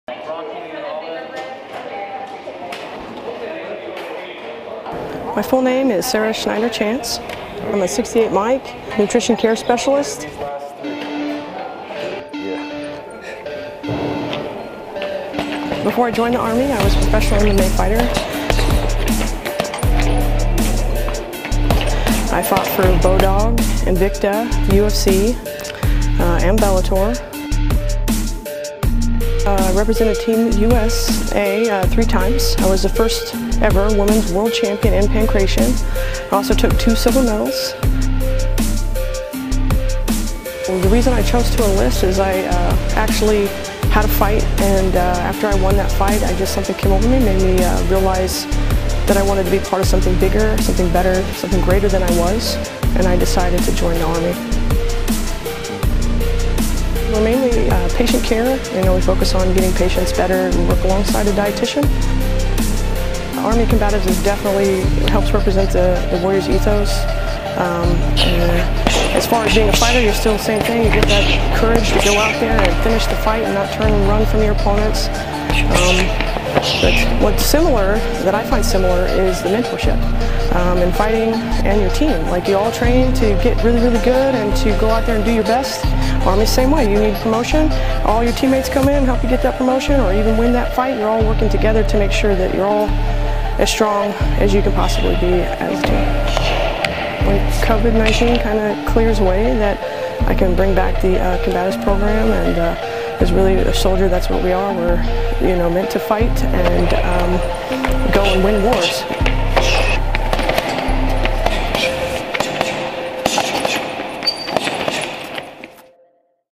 MMA fighter turned Soldier discusses past, present
LANDSTUHL, RP, GERMANY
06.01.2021

U.S. Army Sgt. Sarah Schneider Chance once graced the octagon in the world of Mixed Martial Arts. She now finds purpose in uniform, serving her country in a mission "bigger than herself."

Film Credits: Video by Marcy Sanchez
Landstuhl Regional Medical Center

Army Medicine
LRMC
StrongerTogether
Target_News_Europe